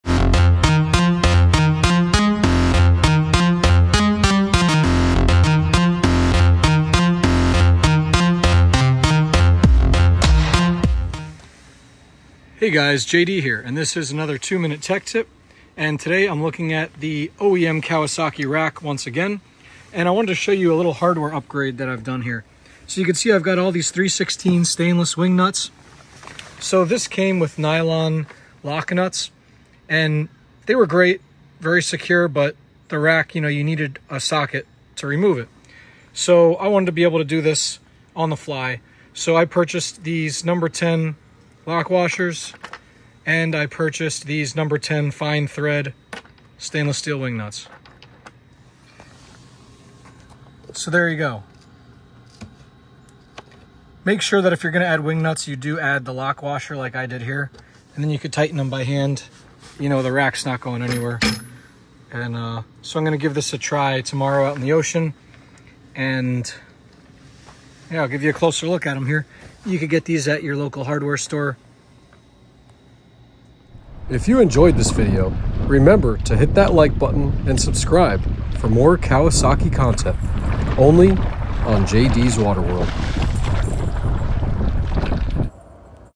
Easy Hardware Fix for the Kawasaki OEM Cargo Rack for the Ultra 160 and Ultra 310 Jet Skis
I like to be able to remove things on the fly, so I added stainless steel wing nuts with stainless lock washers to my OEM Kawasaki Jet Ski cargo rack.